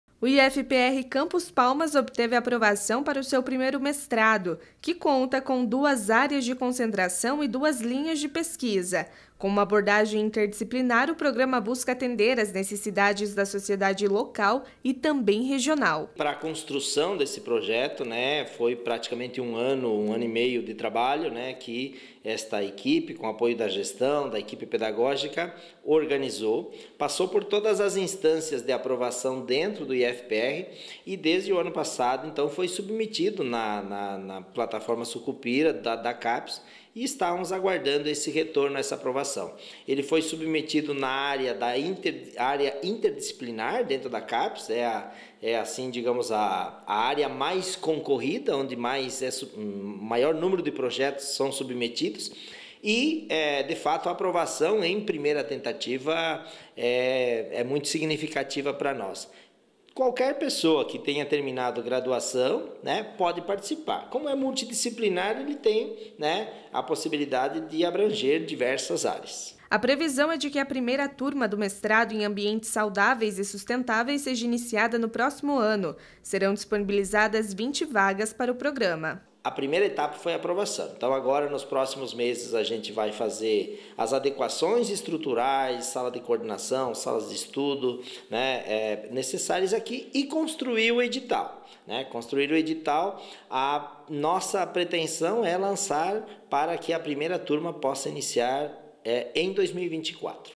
IFPR Campus Palmas tem Mestrado aprovado pela Capes
Educação: No Instituto Federal do Paraná – Campus Palmas, o Curso de Mestrado em Ambientes Saudáveis e Sustentáveis foi aprovado pela CAPES (Coordenação de Aperfeiçoamento de Pessoal e Nível Superior). A proposta para a criação desse mestrado estava em desenvolvimento desde setembro de 2021.